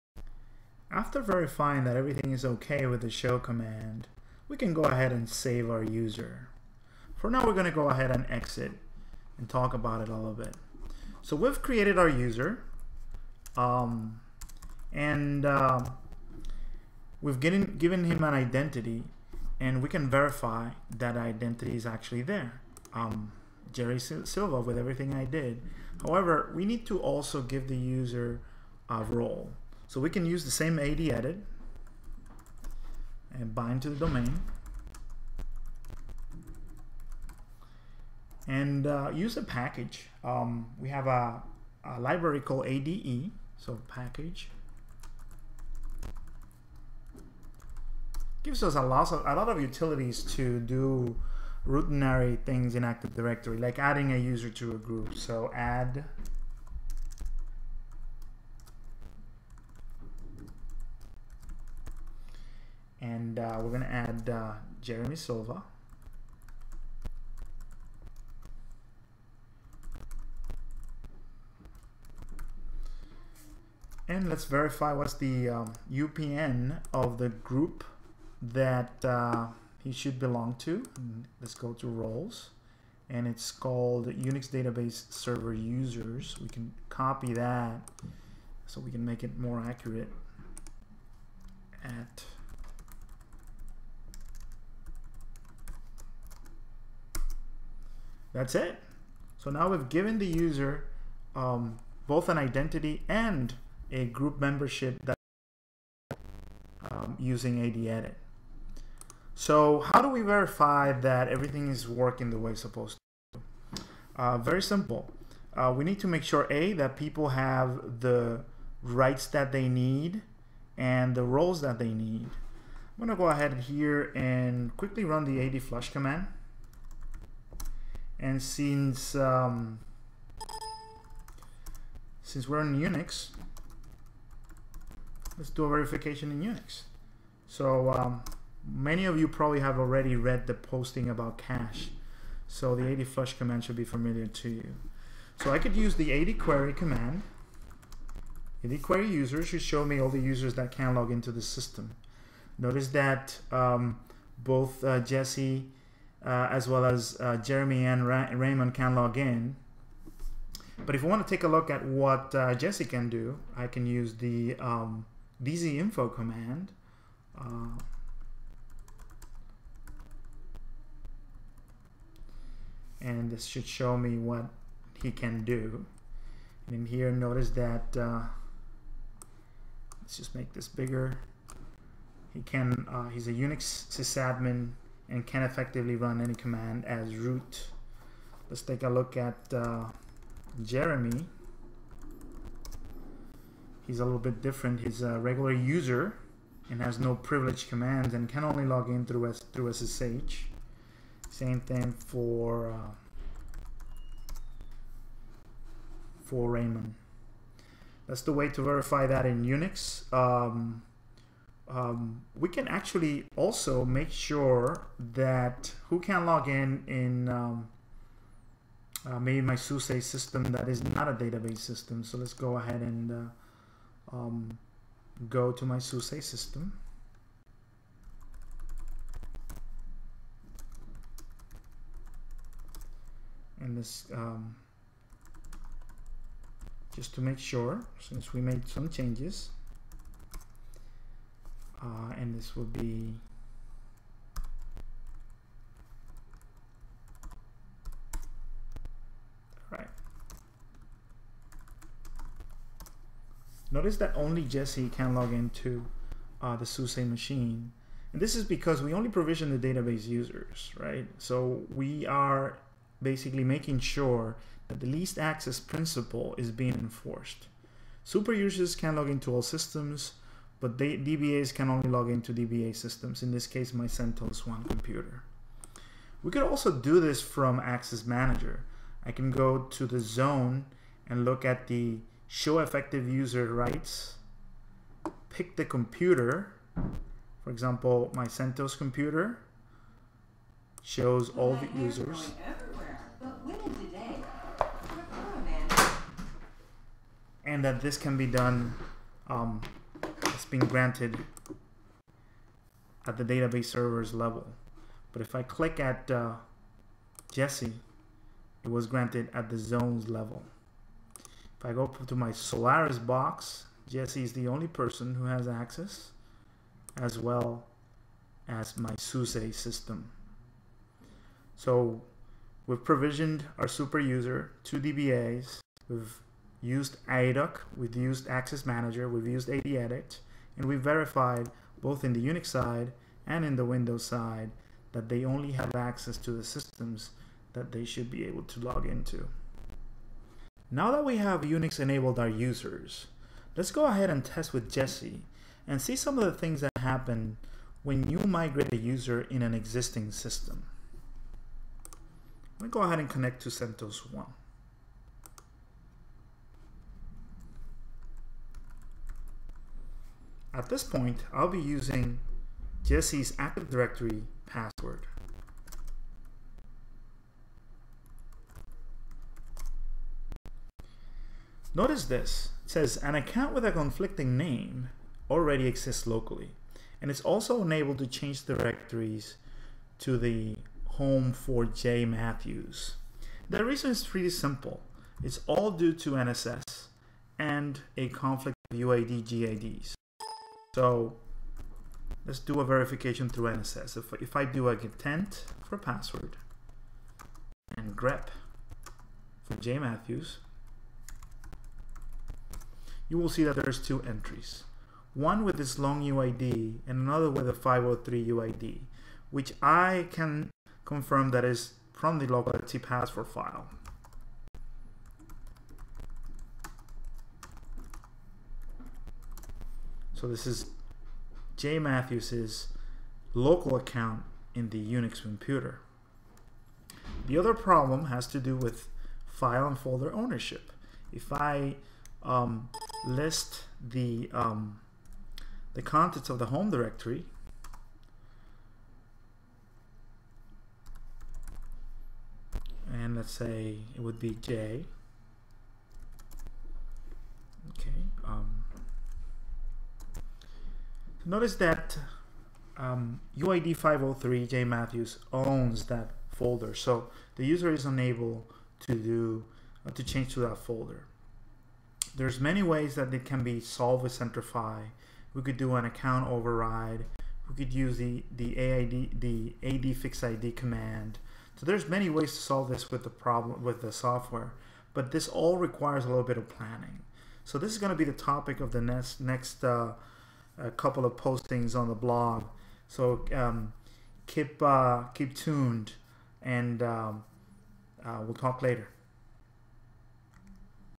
Lab 8b: UNIX Enabling Active Directory users using Centrify
- Finishing up provisioning with adedit
- Verifying user's access (in Linux/UNIX and with Access Manager)
- Uncovering the issue with existing users